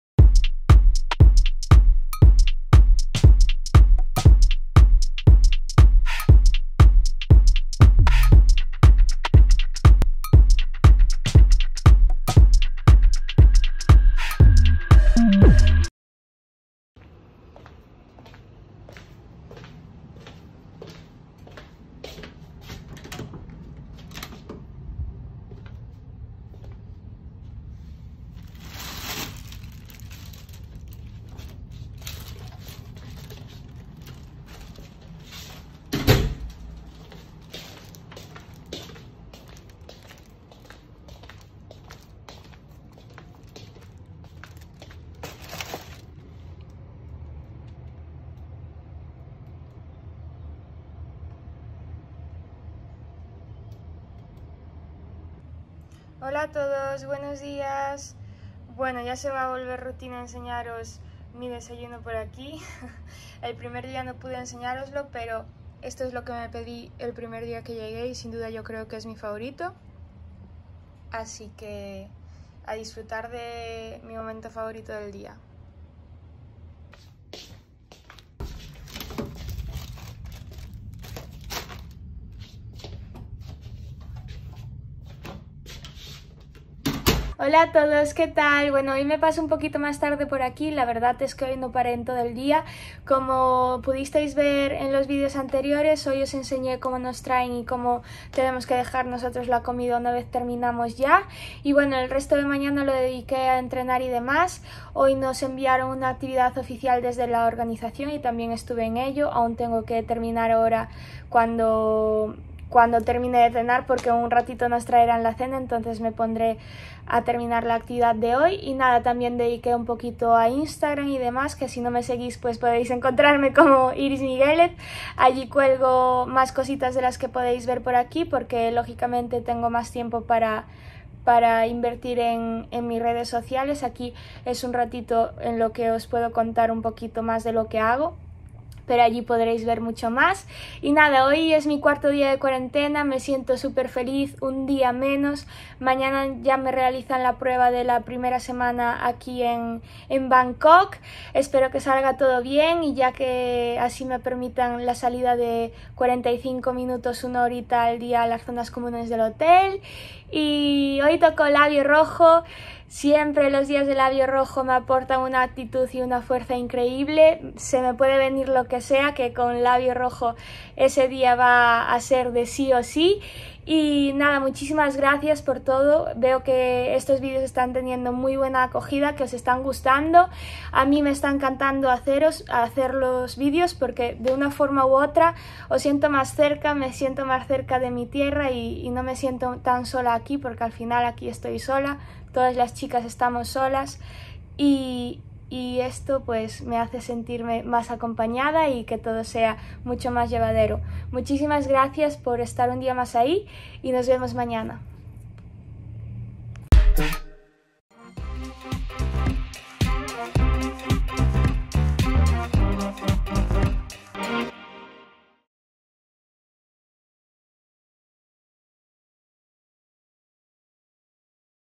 ´Miss diarios´: El día a día de una modelo en Tailandia. Día 4
Cuarto día de cuarentena para Iris Miguélez en Bangkok, donde pasa sola cada día. Eso sí, en su vídeo de hoy manda un mensaje a todos sus seguidores, gracias a los que se siente más arropada. Además, hoy sorprende con un maquillaje diferente, con un toque de color que alegra a cualquiera durante la pandemia.